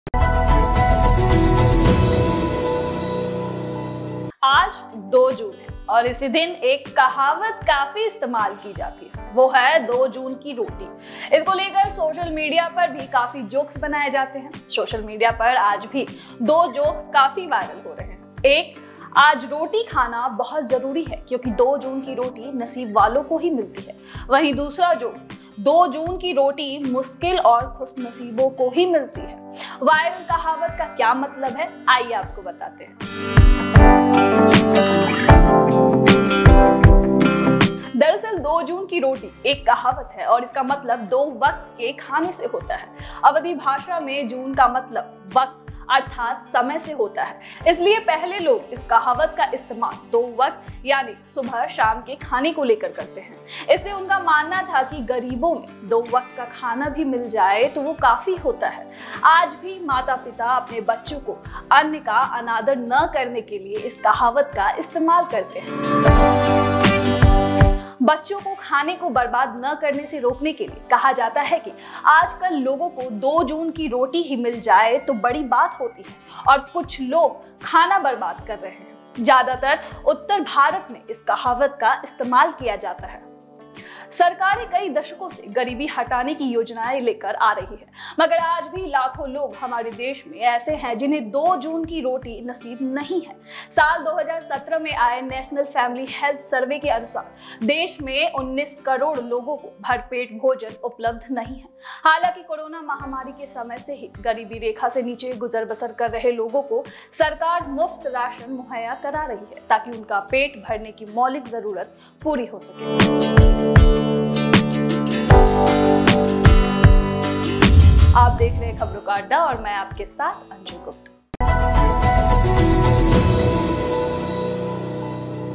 क्यों कहते हैं 2 June Ki Roti ?
2 June ki roti is a proverb and it means eating for 2 times. In the period language, June means time i.e. time. That's why earlier people used this saying about two times i.e. morning and evening food. It is enough if you get it.. even today parents use this proverb to tell their children not to disrespect food. To prevent children from wasting food, it is said that nowadays it is a big deal for people to get bread for two days and some people are wasting food. Mostly this saying is used in North India.
Governments have been coming up with schemes to remove poverty for many decades, but even today there are lakhs of people in our country who do not get bread on June 2. According to the National Family Health Survey, which came in the year 2017, 19 crore people in the country do not have enough food. Is getting it done so that the basic need of filling their stomach can be fulfilled.

KHABRON KA ADDA is a high-quality investigative and questioning journalism with news, analysis.

KHABARON KA ADDA | khabron ka adda | Hindi News | Sports News | Lok Sabha News